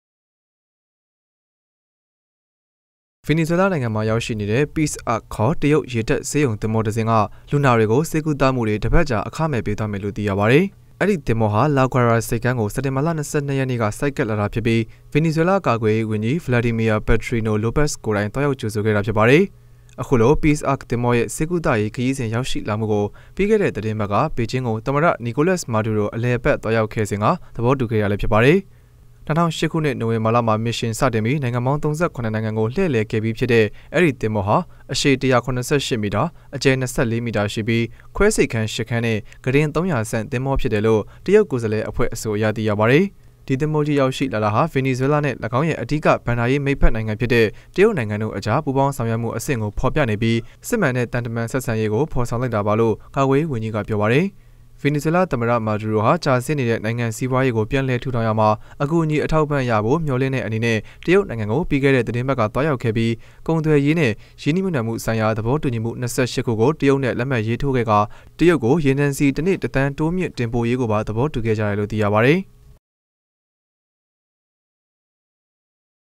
တရုတ္ေဆးရံုသေဘၤာက က်န္းမာေရးေစာင္႔ေရွာက္မႈမ်ား အခမဲ႔ ေပးမည္
ဗင္နီဇြဲလားတြင္ေရာက္ရွိေနသည့္ တရုတ္ေဆးရံုသေဘၤာက က်န္းမာေရးေစာင္႔ေရွာက္မႈမ်ား တစ္ပတ္ၾကာ အခမဲ႔ ေပးမည္ (ရုပ္သံ)

ဗင္နီဇြဲလားႏိုင္ငံမွာေရာက္ရွိေနတဲ႔ Peace Ark (ေခၚ) တရုတ္ေရတပ္ေဆးရံုသေဘၤာတစ္စင္းက လူနာ ေတြကို ေဆးကုသမႈေတြ တစ္ပတ္ၾကာအခမဲ႔ေပးသြားမယ္လို႔ သိရပါတယ္။